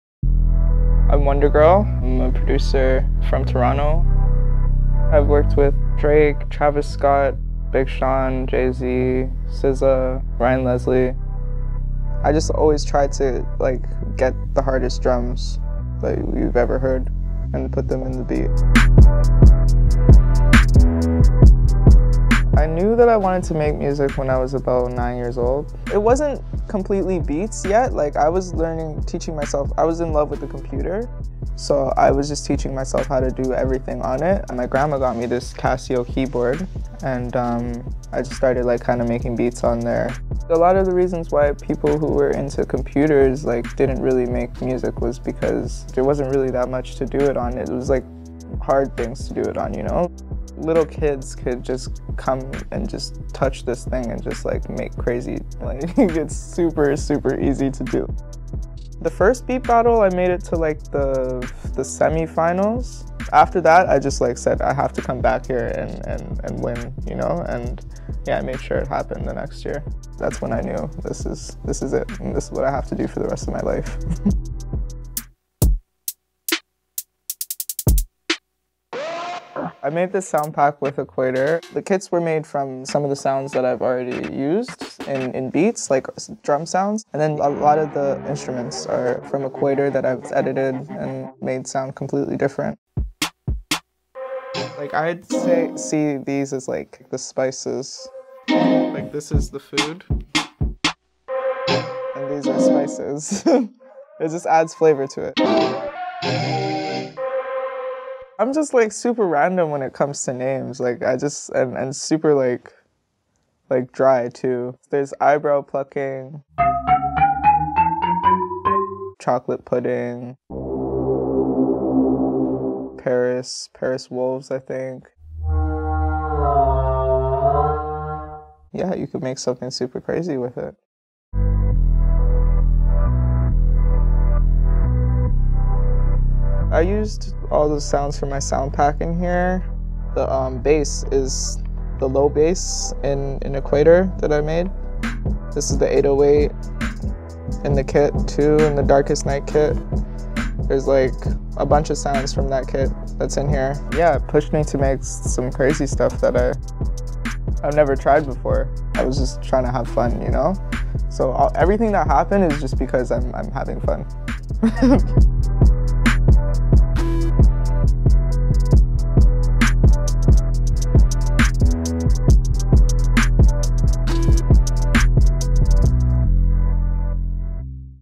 Soundpack Stories: WondaGurl's Distorted Bliss
Capture the gracefully gritty sound of Wondagurl straight outta Toronto with her signature collection of heavy basses, angular beats, and raw urban vibes – get it in the NOISE app now